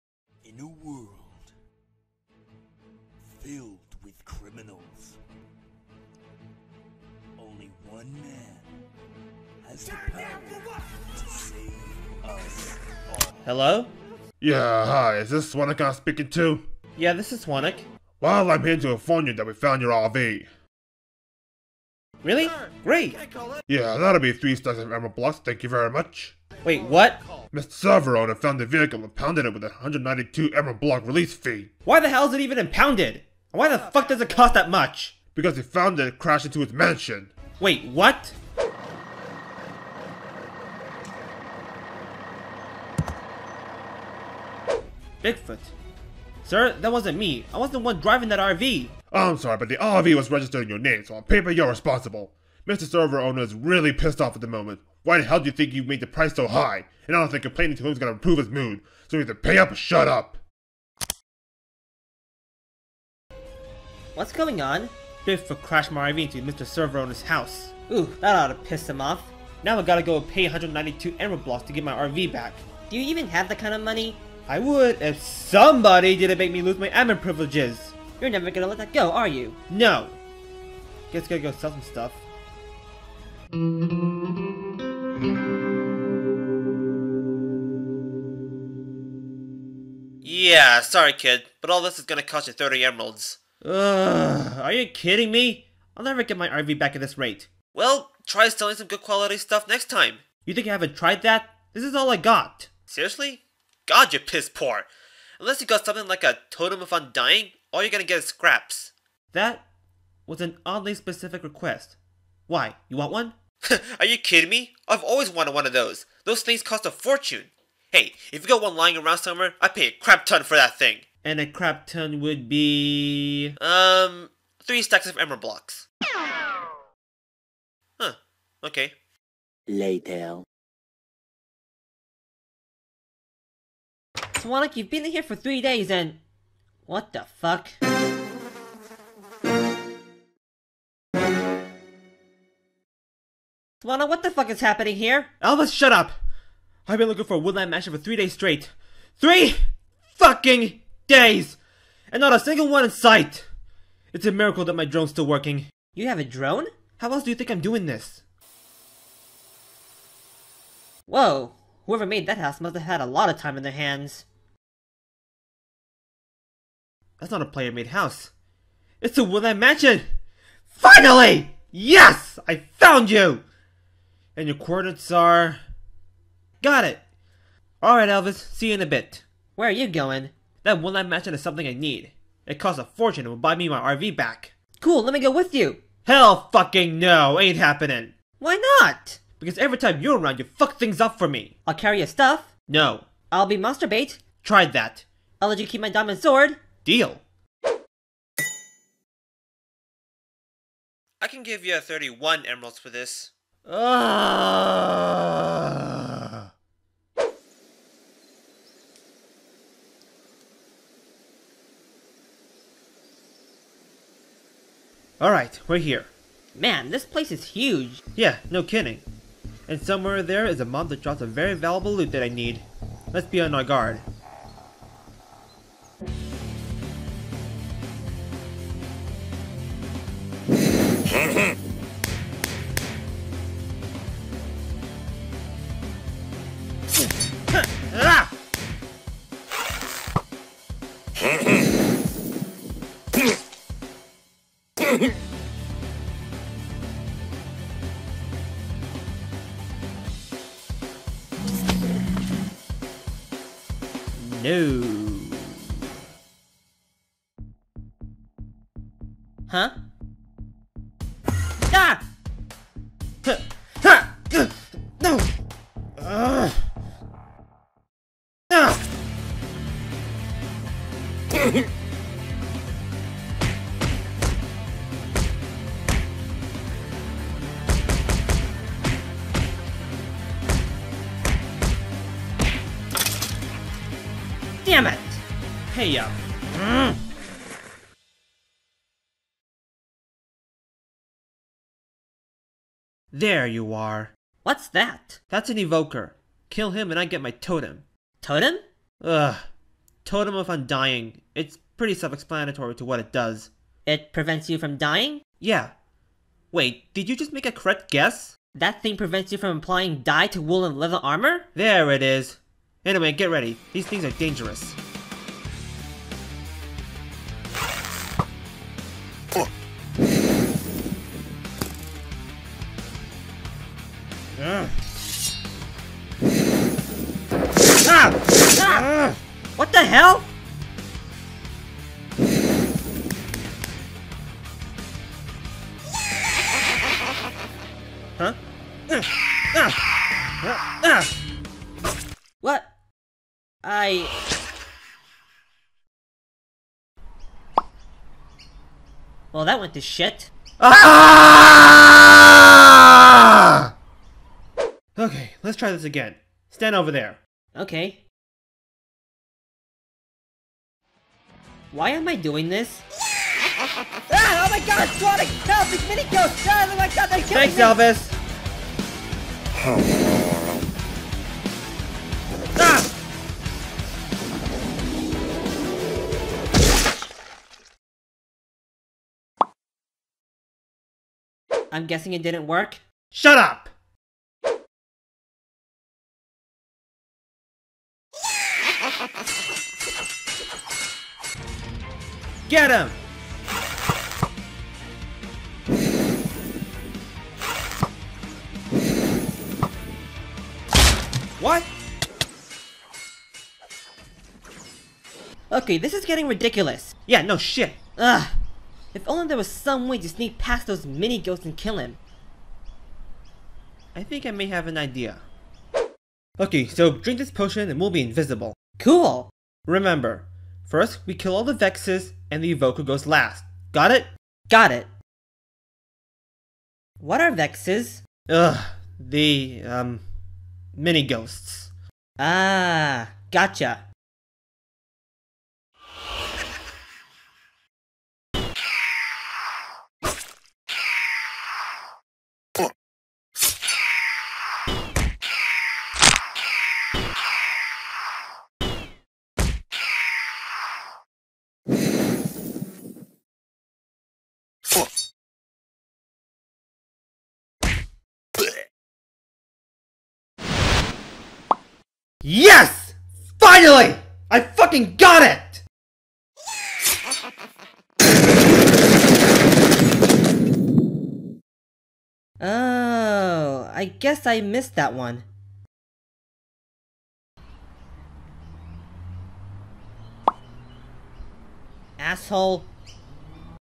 Noobies 2x05 Woodland Mansion (Mid-Season Finale)
Hey Everybody Swonic here and today Bigfoot crashes Swonic's RV into Mr Server Owner's Mansion and the RV ended up impounded. Now Swonic is searching for an item to sell to pay off the impound fee